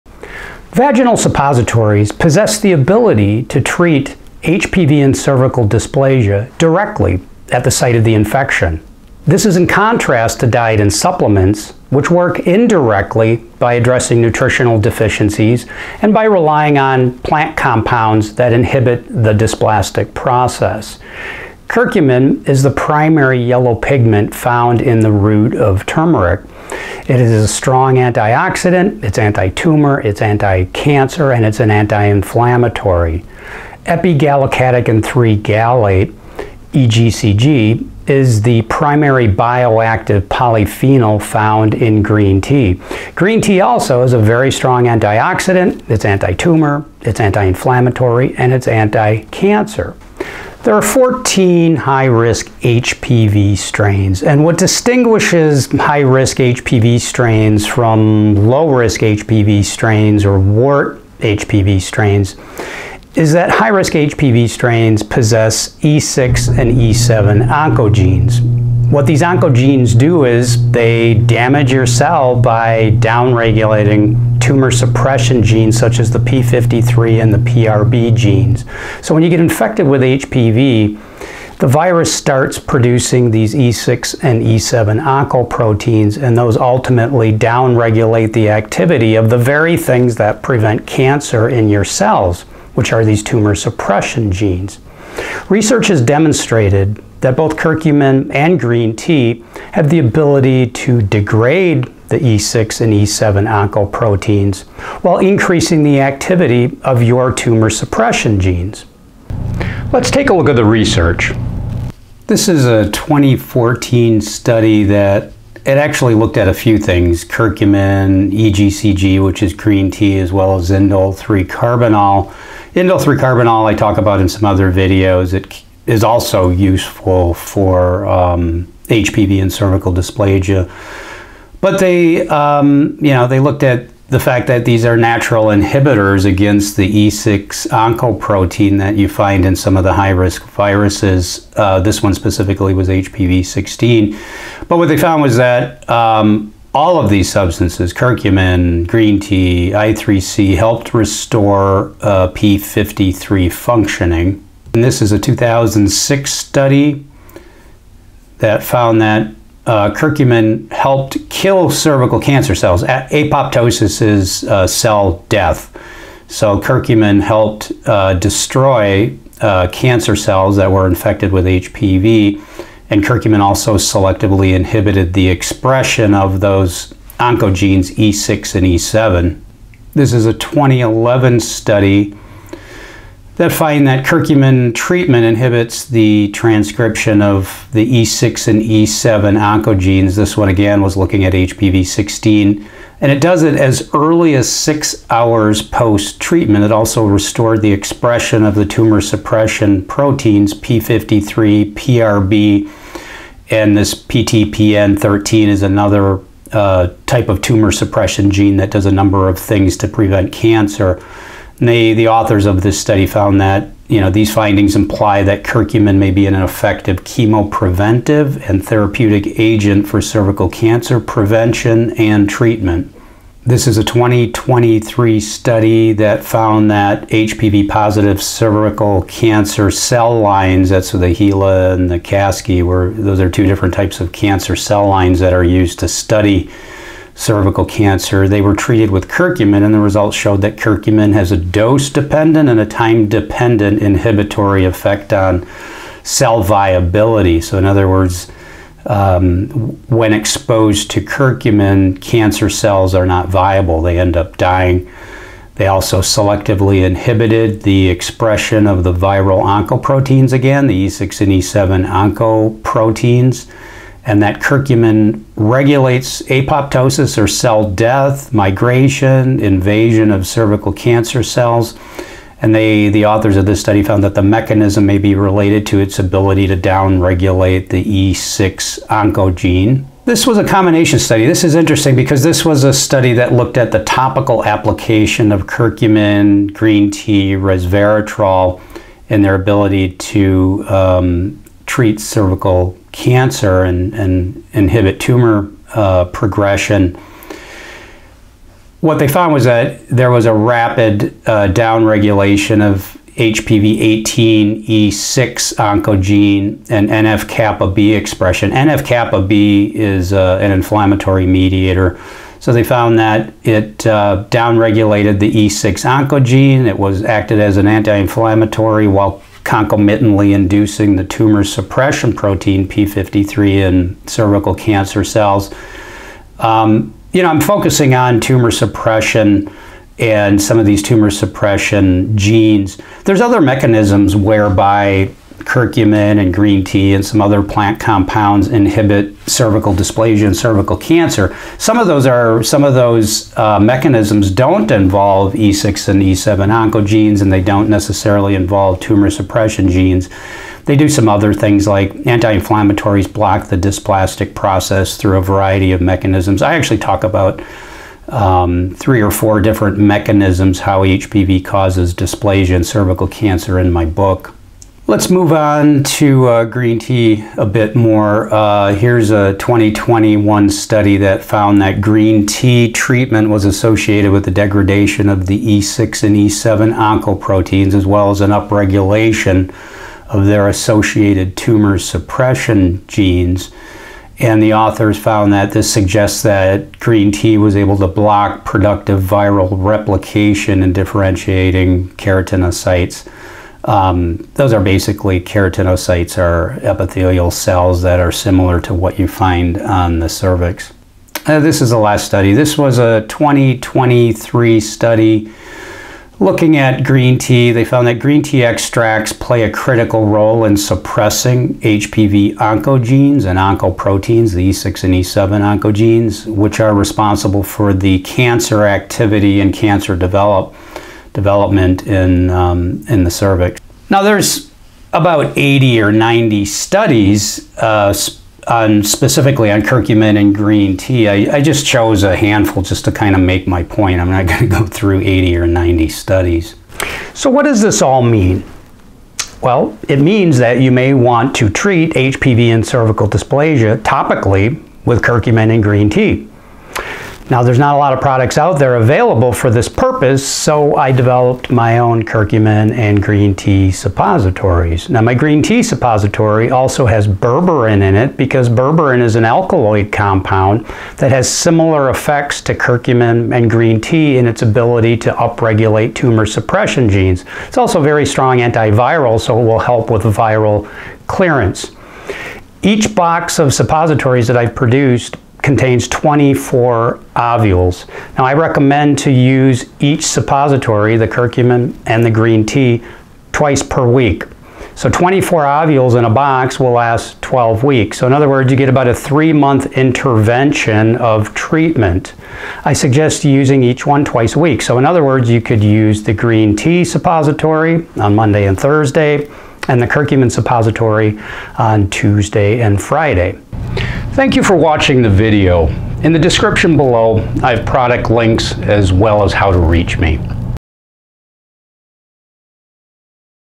Vaginal Suppositories for HPV
Dr. Nick LeRoy has been treating HPV and cervical dysplasia since 1995. Vaginal suppositories can address HPV and dysplasia at the site of the infection. In this video, Dr. LeRoy discusses the rationale of suppositories as well as the research that supports their use.

Register with code: nleroy

For detailed information on natural treatment options for HPV and cervical dysplasia go